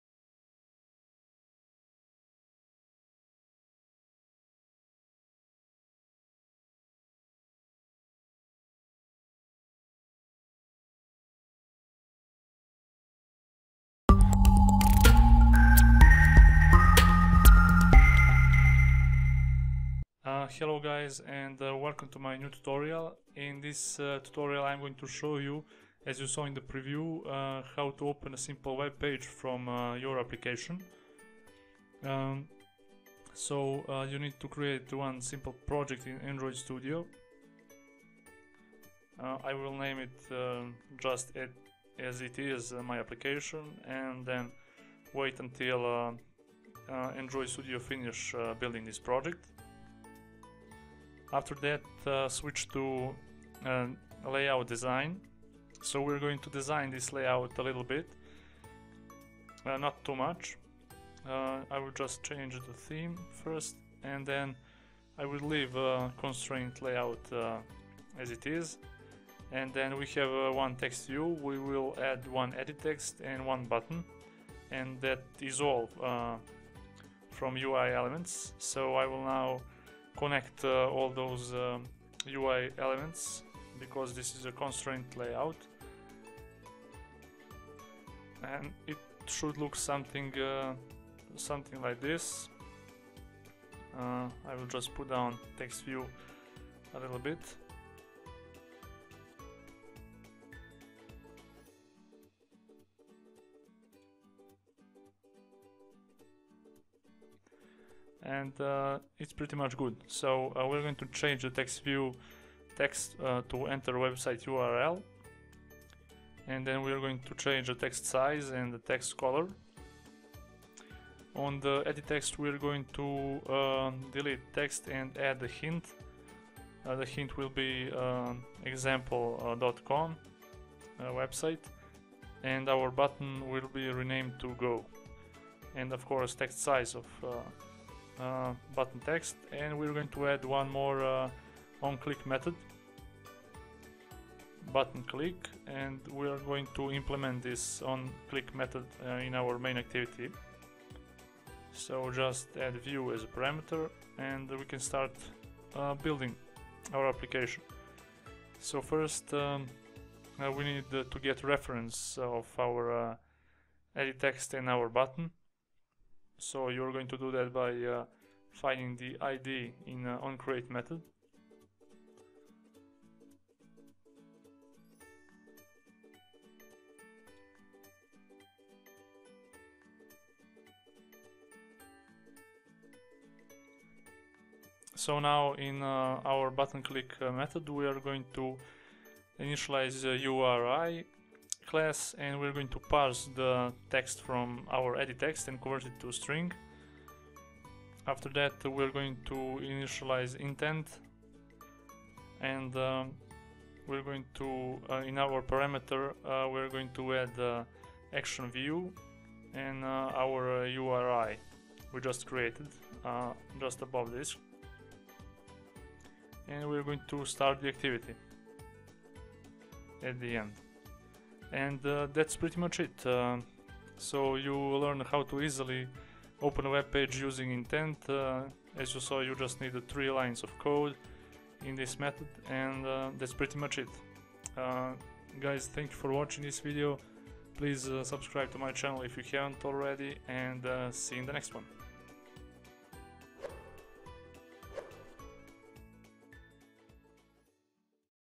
Open Website Page from your Application using Intent | Android Studio
Learn how to simply open a webpage from your application in Android Studio. Watch the whole video to learn how.